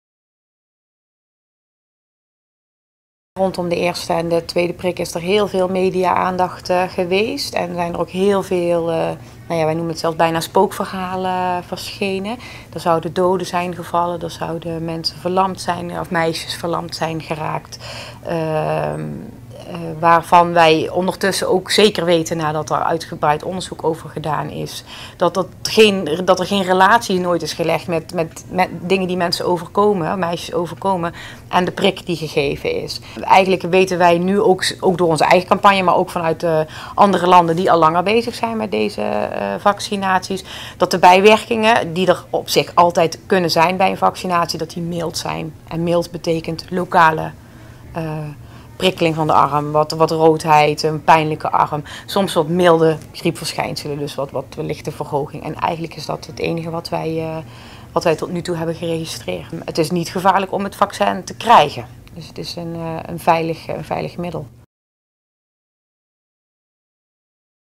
Kun je verlamd raken of dood gaan door de hpv prik?
Een interview met een jeugdarts over eventuele bijwerkingen van de hpv prik tegen baarmoederhalskanker. De juiste informatie over de hpv-inenting voor meisjes van 12 jaar vind je op